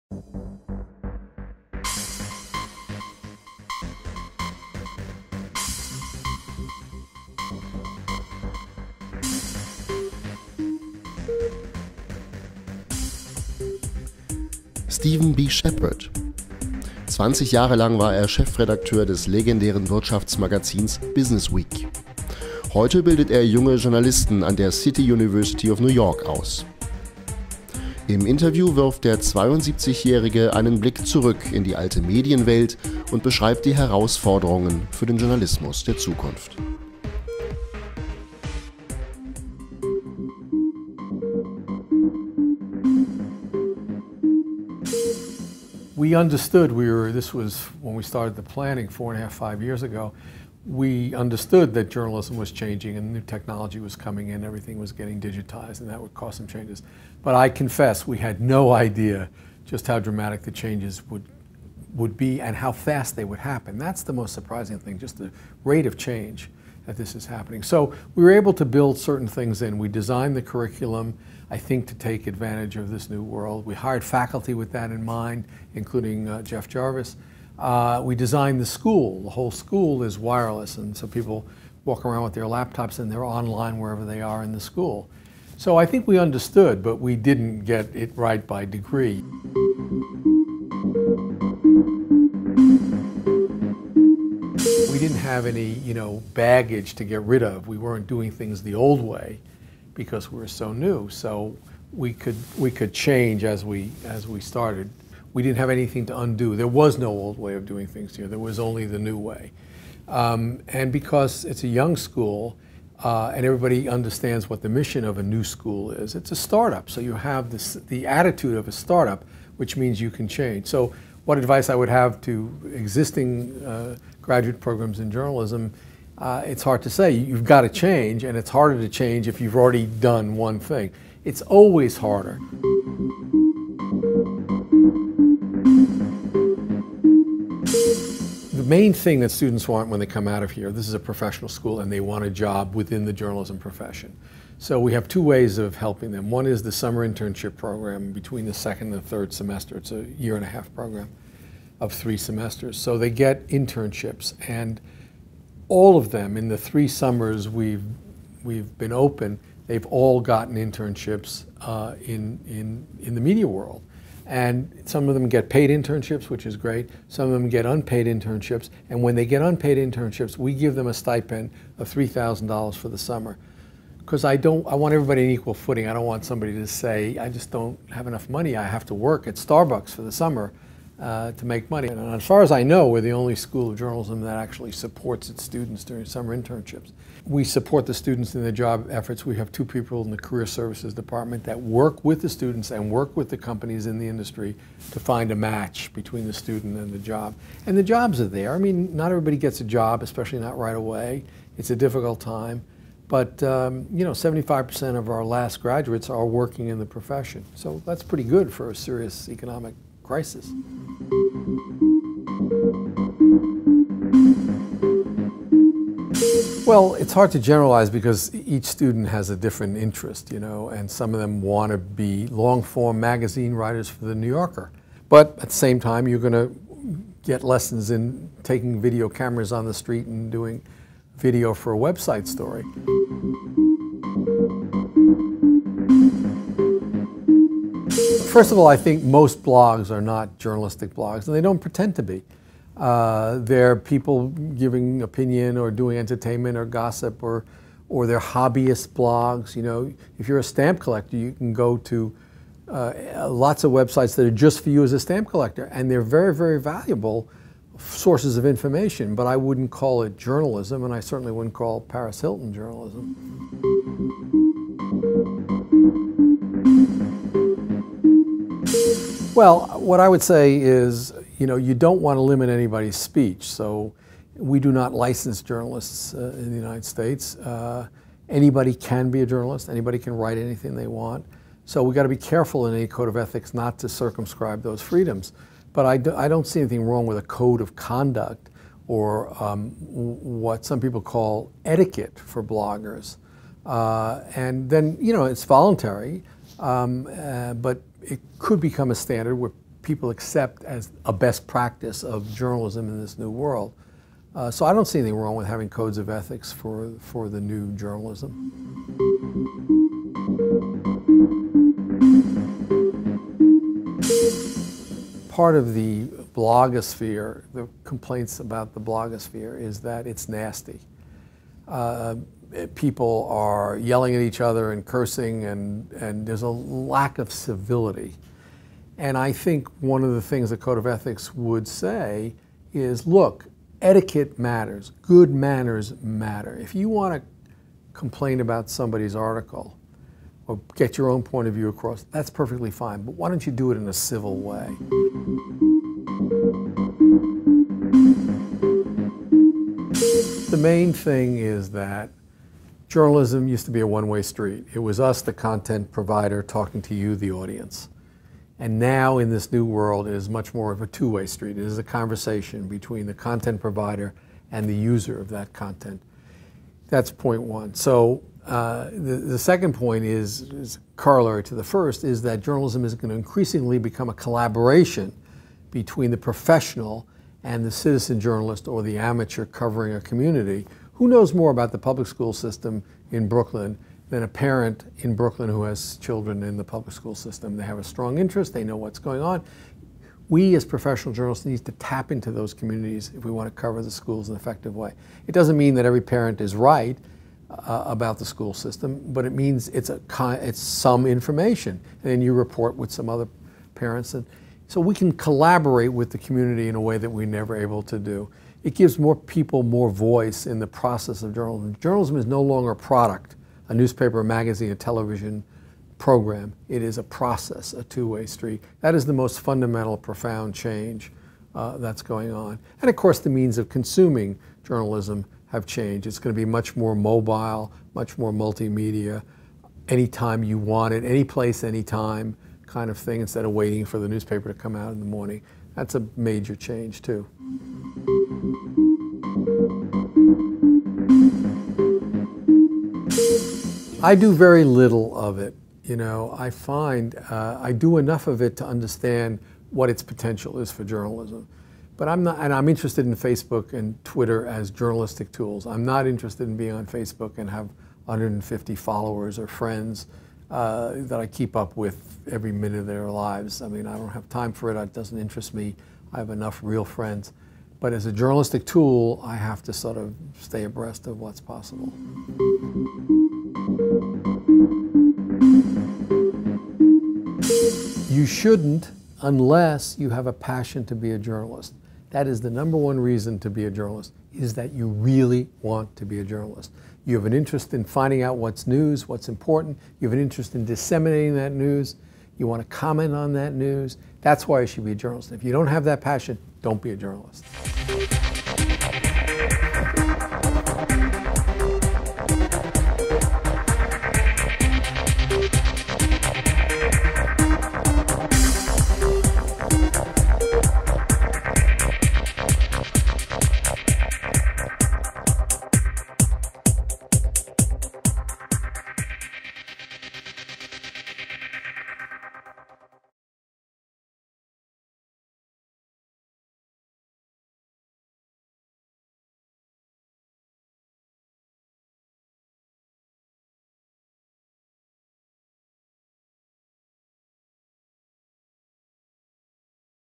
Stephen B. Shepard im VOCER-Interview
Stephen B. Shepard gehört zu den Urgesteinen der amerikanischen Medienwelt: Über 20 Jahre lang war er Chefredakteur des legendären Wirtschaftsmagazins "Business Week". Heute gibt er seine Erfahrung an die Journalistik-Studenten der City University of New York weiter. Im VOCER-Interview analysiert Shepard die neuen Anforderungen an den Journalismus. Eine VOCER-Produktion im Rahmen der Interviewreihe "Digitale Mediapolis".

Autoren: Stephan Weichert, Leif Kramp
Kamera: Iris Ockenfels
Schnitt/ Mischung: Christoph Brüggemeier
Sprecher: Holger Hettinger
Regie: Iris Ockenfels
Mitarbeit: Pia Gurgel, Christin Weigelt
Redaktionsleitung: Stephan Weichert